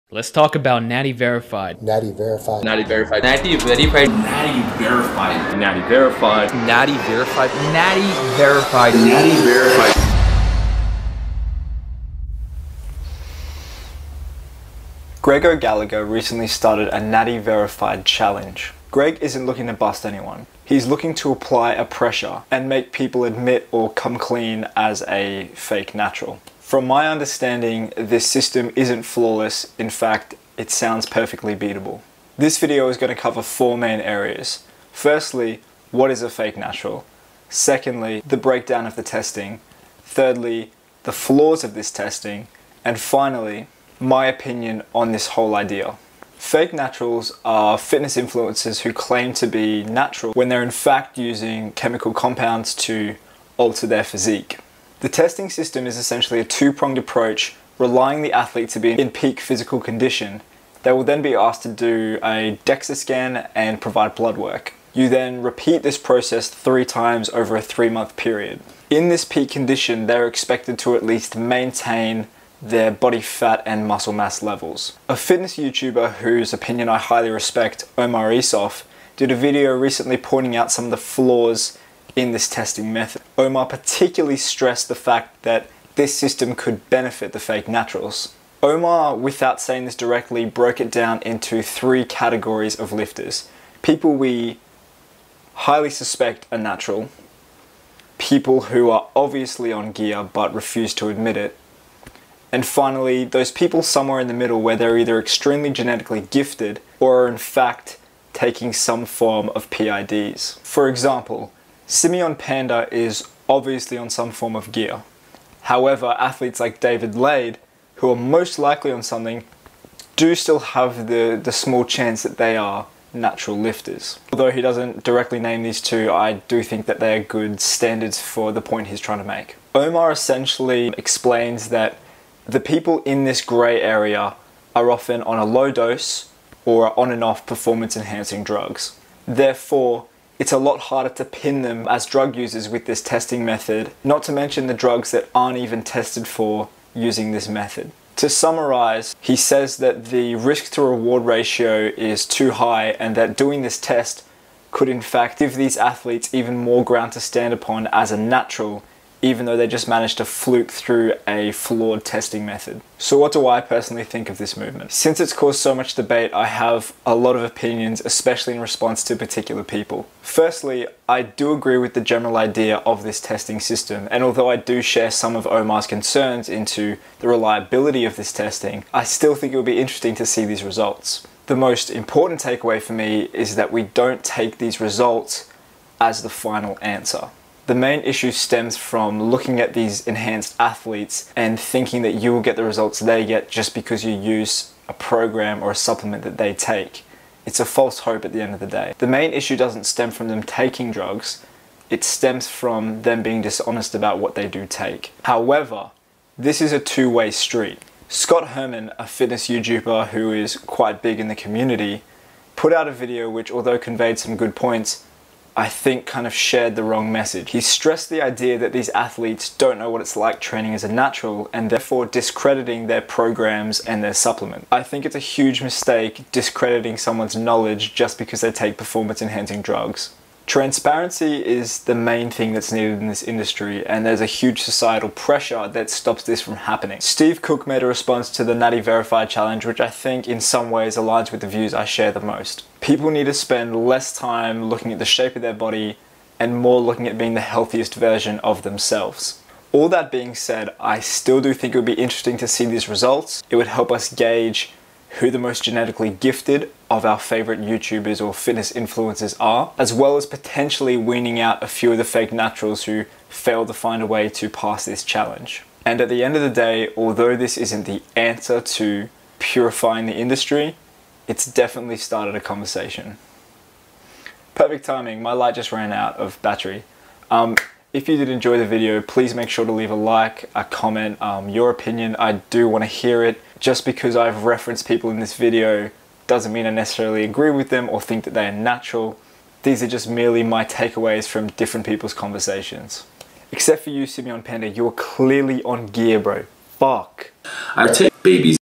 Natty Verified: Everything Explained
This is a summary of the general ideas thrown around about the NV movement. This is aimed at being a response video stating my own educated opinion.

Make sure to leave your opinions down below and 1 like=1 baby protected from Steve Cook